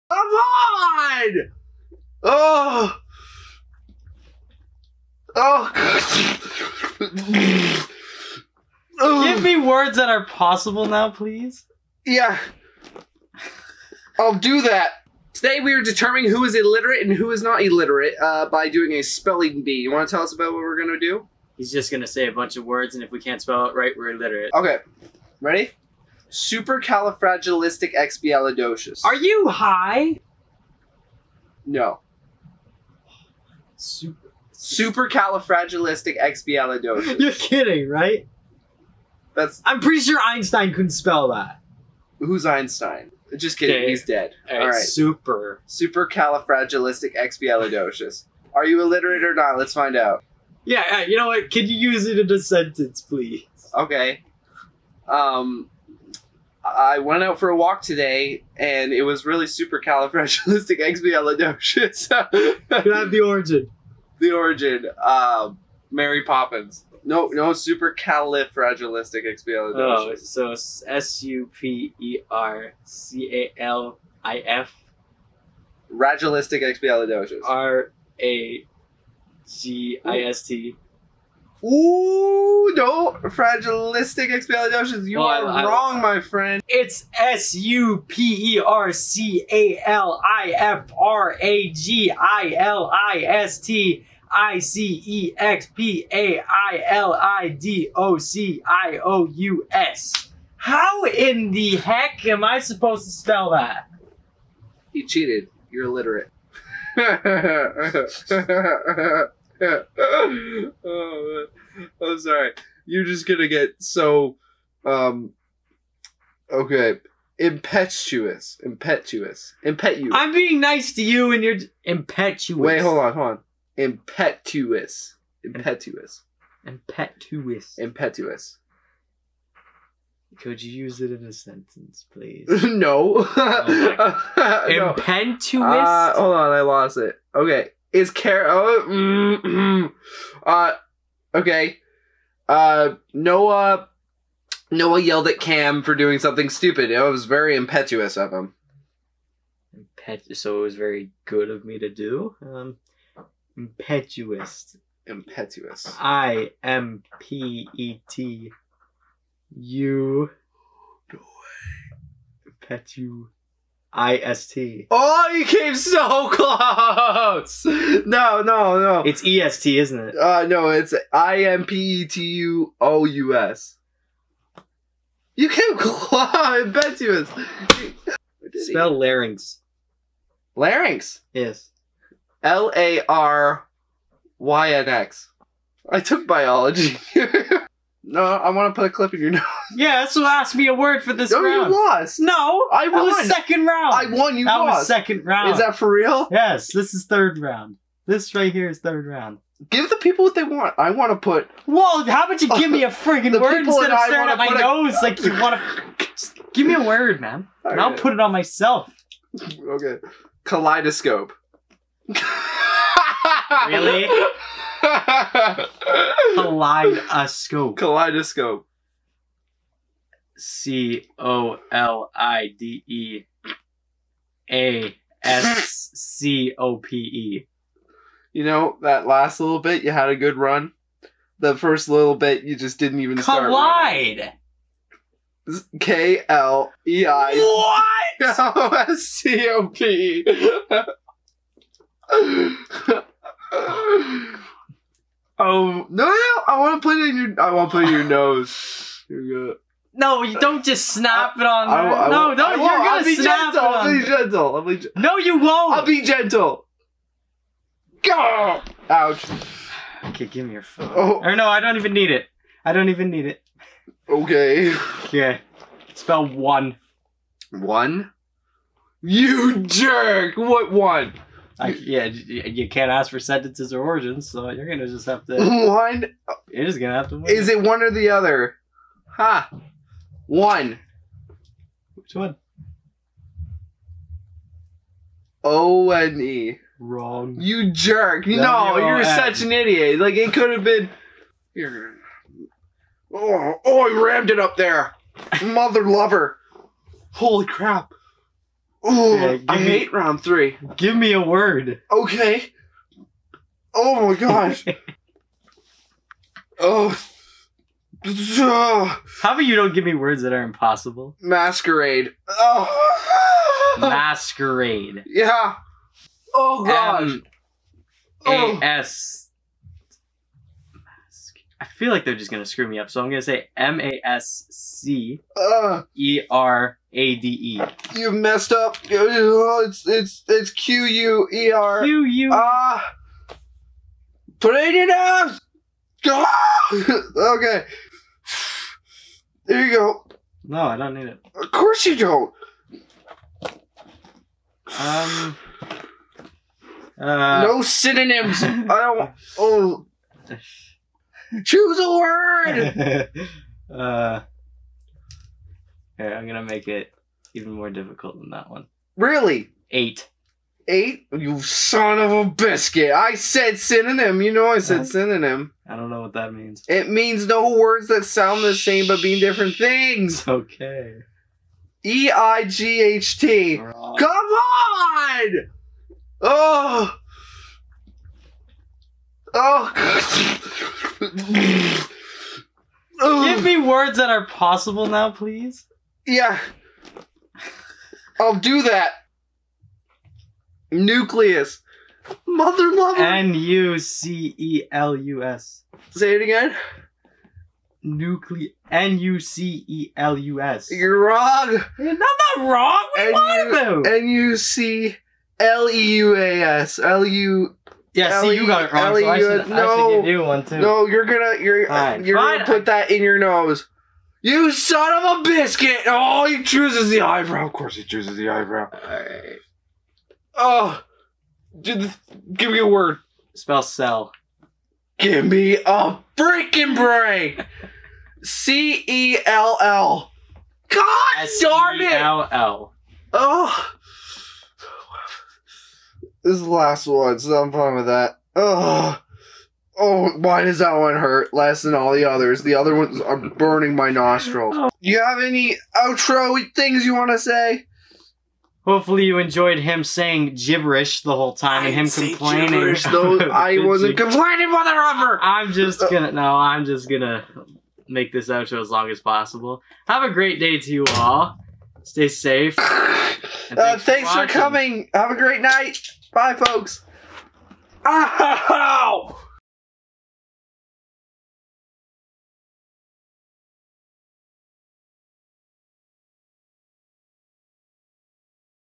From Tears To Screams To Complete Illiteracy I IBB
IBB goes a different way with ramming close pins up the nose in an effort to determine complete illiteracy!

More games, more fun, more challenges to come!